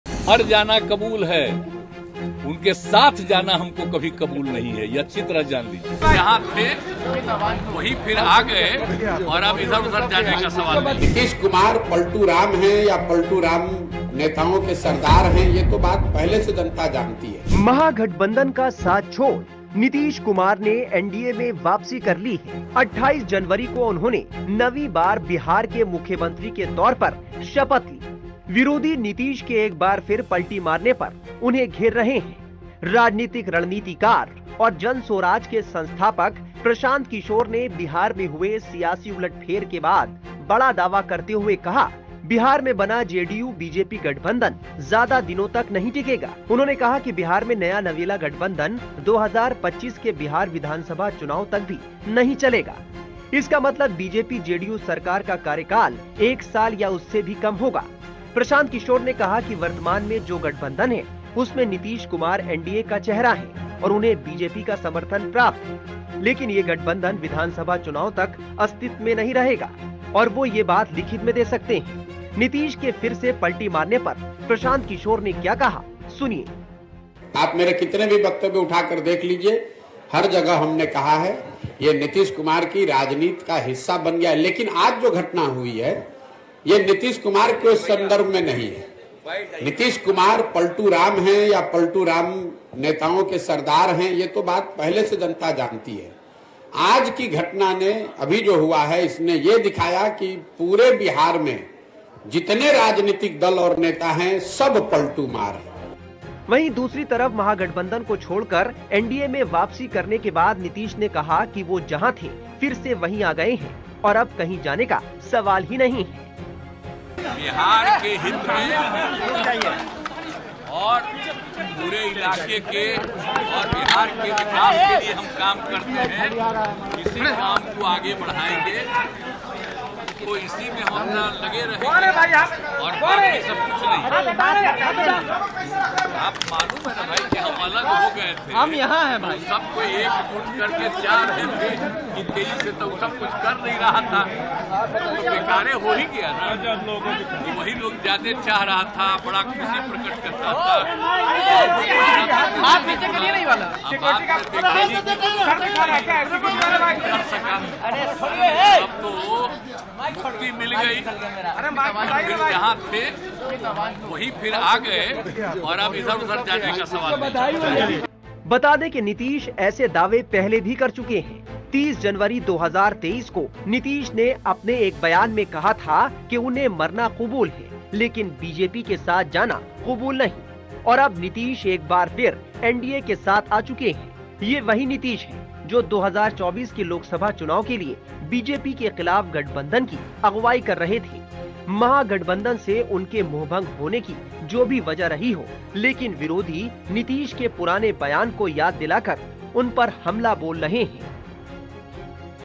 Prashant Kishor on Nitish: नीतीश की NDA में वापसी पर क्या बोले प्रशांत किशोर ?
Prashant Kishor on Nitish:महागठबंधन का साथ छोड़ नीतीश कुमार ने NDA में वापसी कर ली है.28 जनवरी को उन्होंने 9वीं बार बिहार के मुख्यमंत्री के तौर पर शपथ ली. विरोधी नीतीश के एक बार फिर पलटी मारने पर उन्हें घेर रहे हैं.राजनीतिक रणनीतिकार और जनसुराज के संस्थापक प्रशांत किशोर ने बिहार में हुए सियासी उलटफेर के बाद बड़ा दावा करते हुए कहा कि बिहार में बना जेडीयू -बीजेपी गठबंधन ज्यादा दिनों तक नहीं टिकेगा. उन्होंने कहा कि बिहार में नया-नवेला गठबंधन 2025 के बिहार विधानसभा चुनाव तक भी नहीं चलेगा.